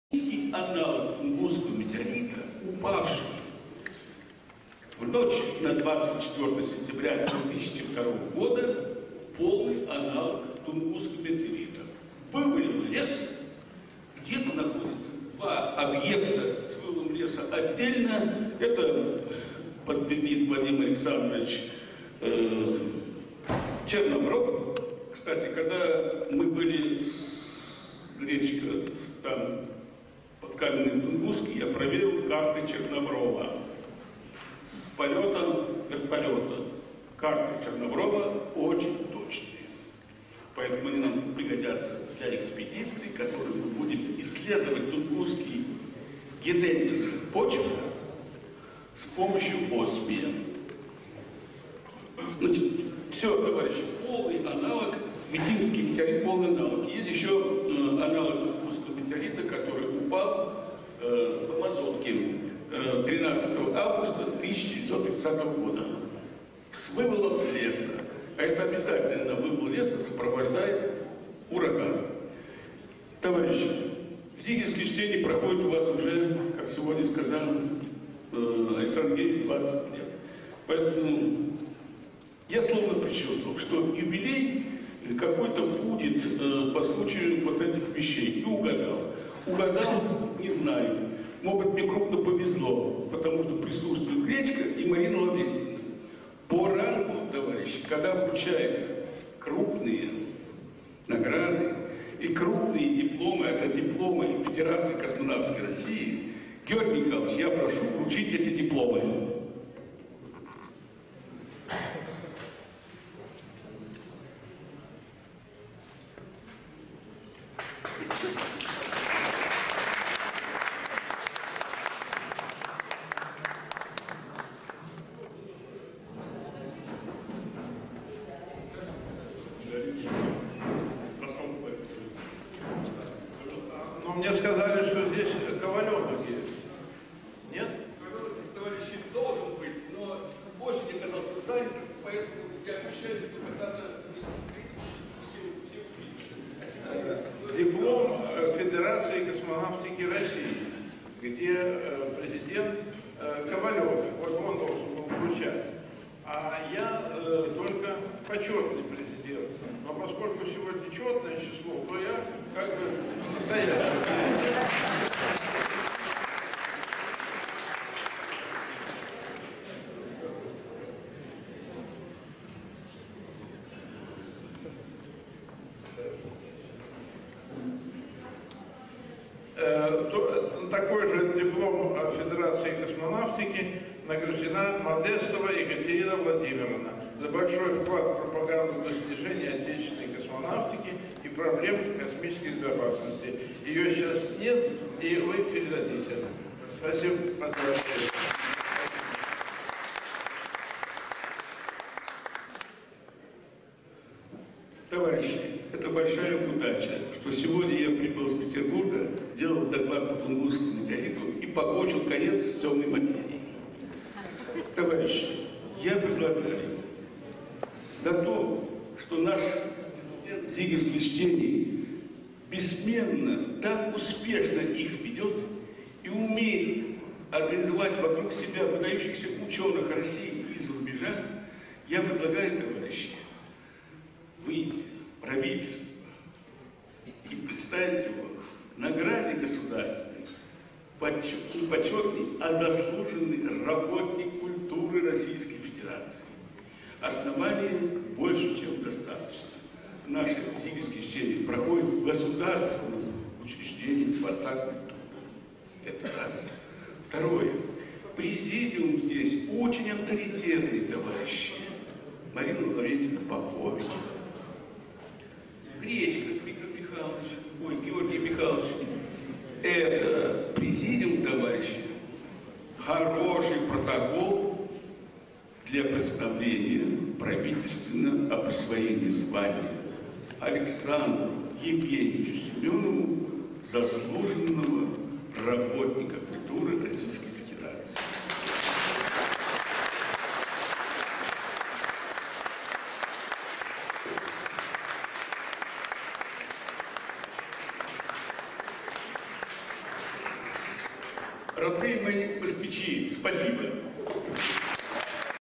Зигелевские чтения №33 (22.11.2008 г.) Евгений Измайлович Боровков
Международная общественно-научная конференция "Зигелевские чтения", организованная Ассоциацией "Экология Непознанного" 22 ноября 2008 года. Евгений Измайлович Боровков, президент Международной Академии «Информация, связь, управление в технике, природе, обществе», директор НИИ астробиологических проблем и космической безопасности им. С.И.Репьева, Владимир Васильевич Коваленок, президент Федерации космонавтики России, дважды Герой Советского Союза, летчик-космонавт СССР, профессор, генерал-полковник авиации, Виталий Васильевич Довгуша, директор, Юрий Николаевич Егоров, профессор, заместитель директора по научной работе НИИ промышленной и морской медицины, академики МАИСУ (С.-Пб). 100 лет Тунгусскому космическому феномену